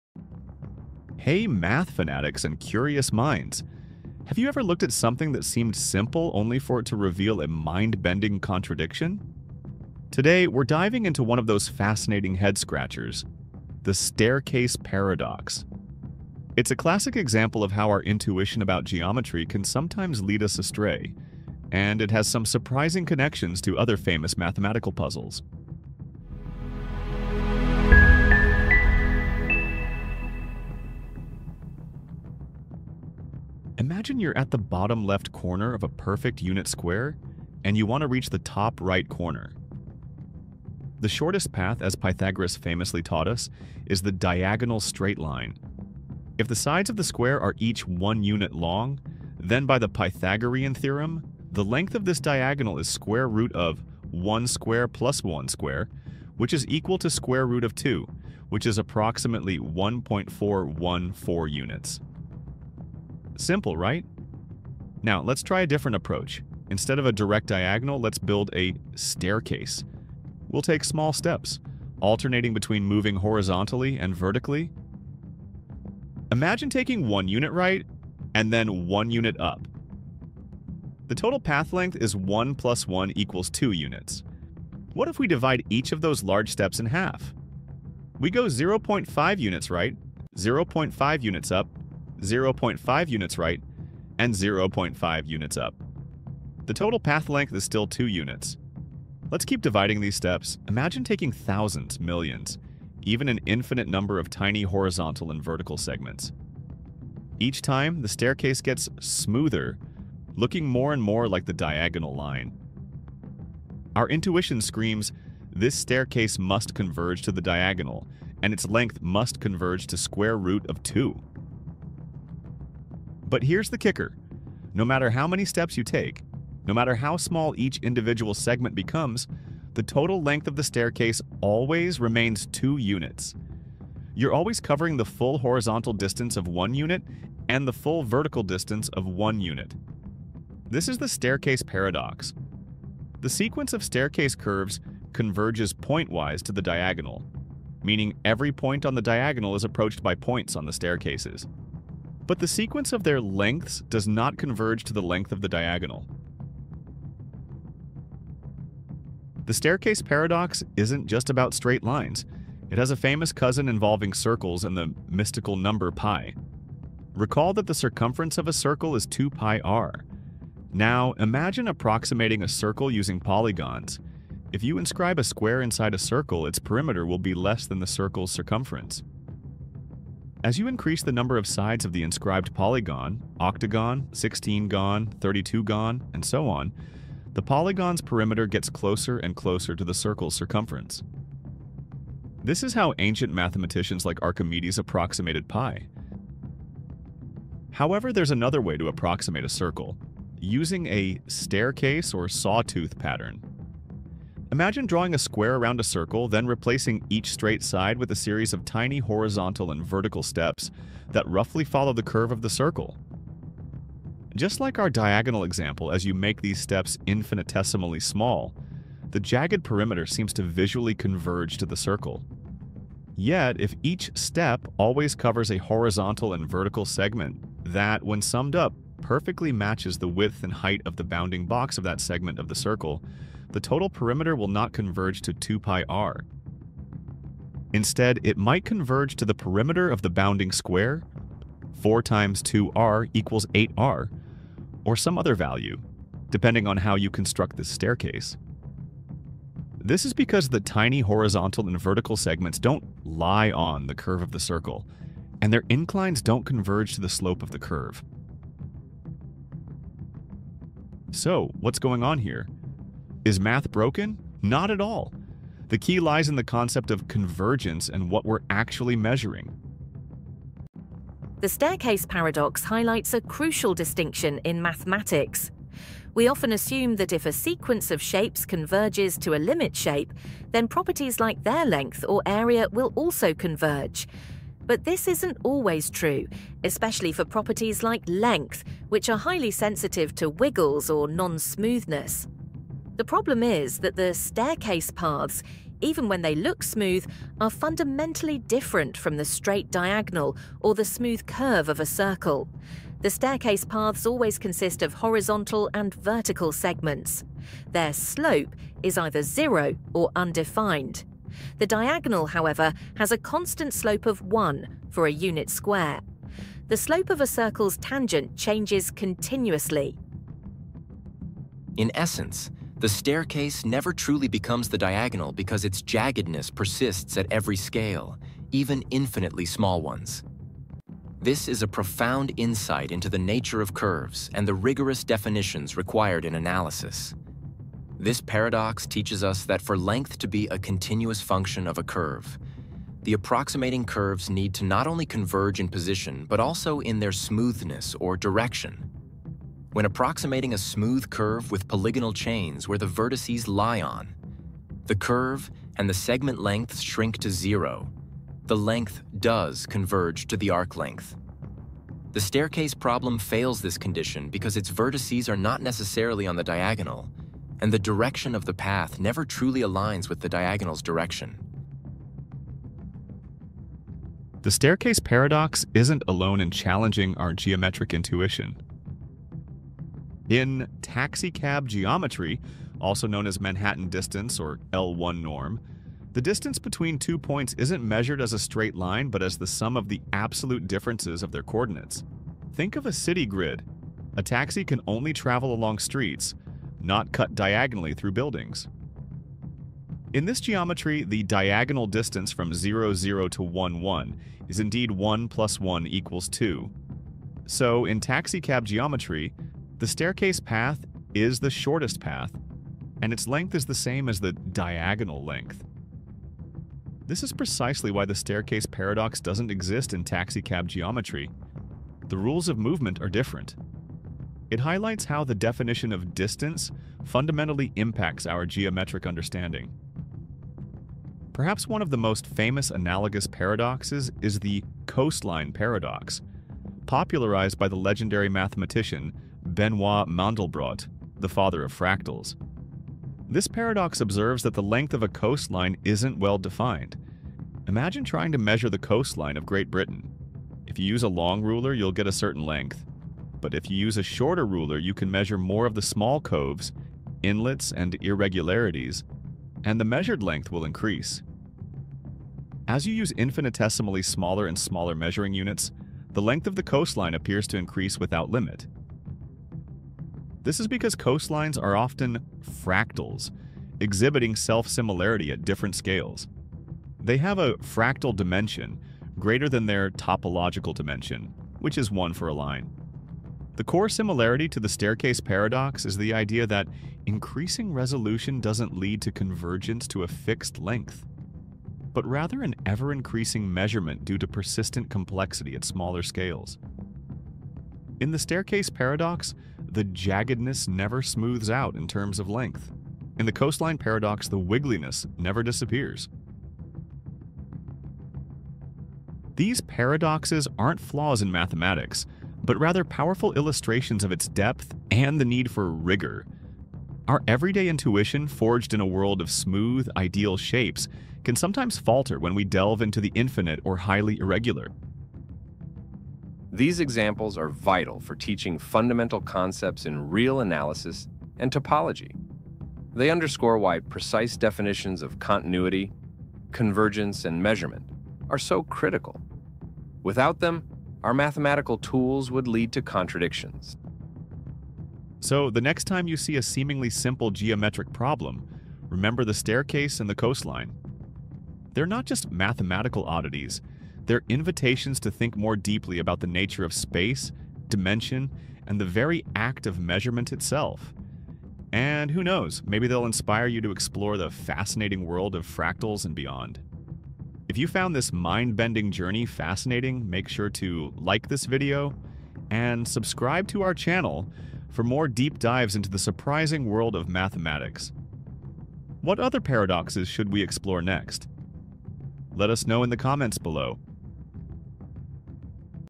The Staircase Paradox: When Math Plays Tricks on Our Intuition
Have you ever looked at something that seemed simple, only for it to reveal a mind-bending contradiction? Today, we're diving into one of those fascinating head-scratchers: the "Staircase Paradox". It's a classic example of how our intuition about geometry can sometimes lead us astray, and it has some surprising connections to other famous mathematical puzzles.